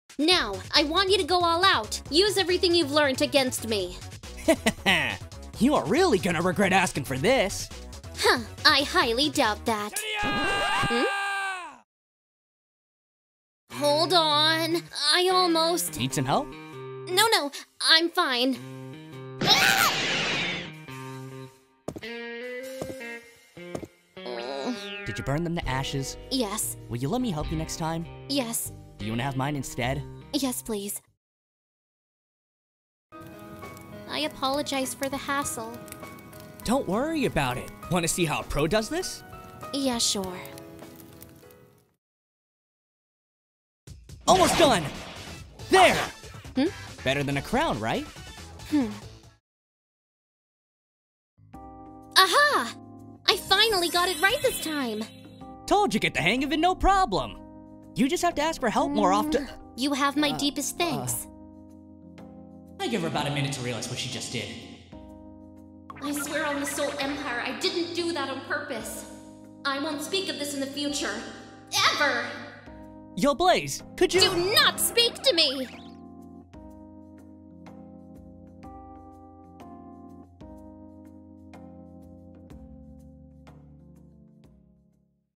Sonic Animation Dub - Sonic & Blaze’s Camping Duo! (Animation Dub Compilation)
Sonic and Blaze camp outside together... as friends right??

Friends in vid:
@InfectiousPink on Twitter

Artist:
SG_Animates on Twitter